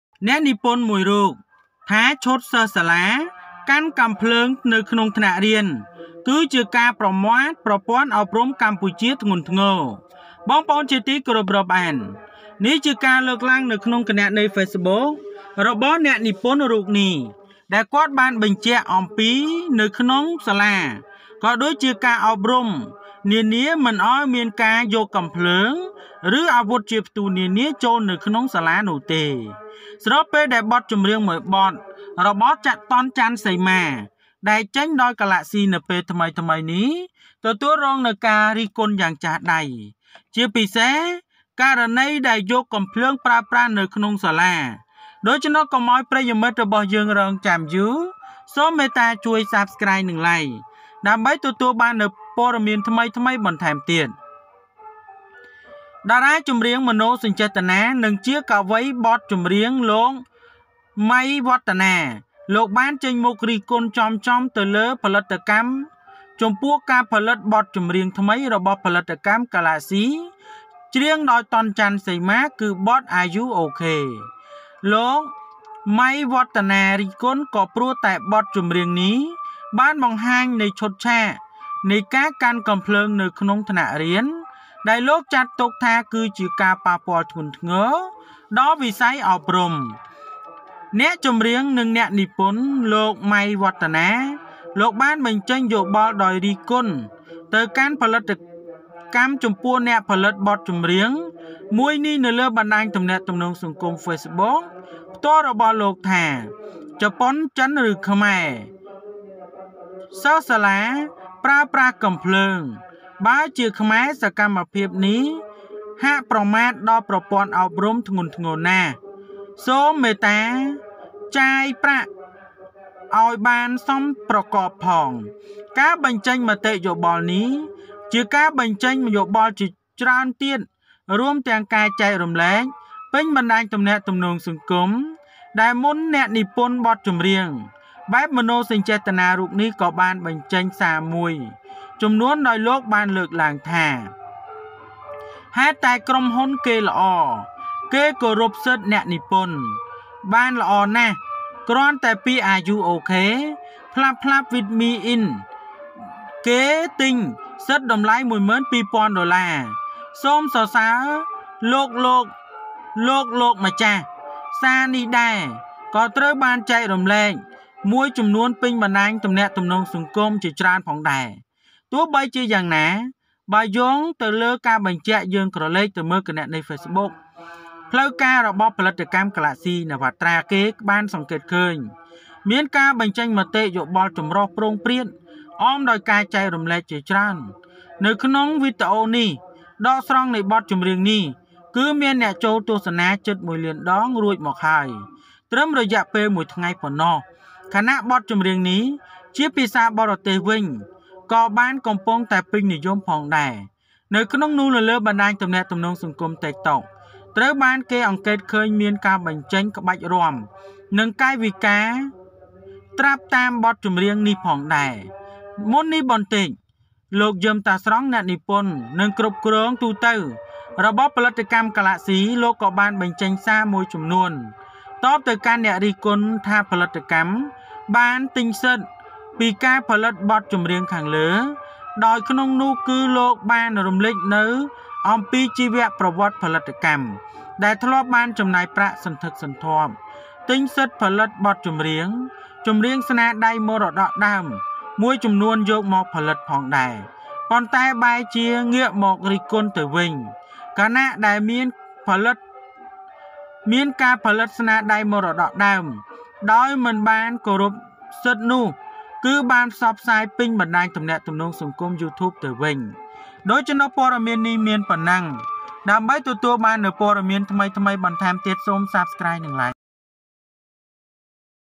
អ្នកនិពន្ធល្បីឈុតសិស្សសាលា កាន់កាំភ្លើងក្នុងថ្នាក់រៀន ជាប្រមាថប្រព័ន្ធអប់រំកម្ពុជា យ៉ាងធ្ង​ន់​ធ្ងរ
អ្នកនិពន្ធមួយរូប ថាឈុតសិស្សសាលា កាន់កាំភ្លើងក្នុងថ្នាក់រៀន គឺជាប្រ​មាថប្រព័ន្ធអប់រំក​ម្ពុជា យ៉ាងធ្ង​ន់​ធ្ងរ An author says schoolgirls carrying guns in the classroom are a serious insult to the Cambodian education system.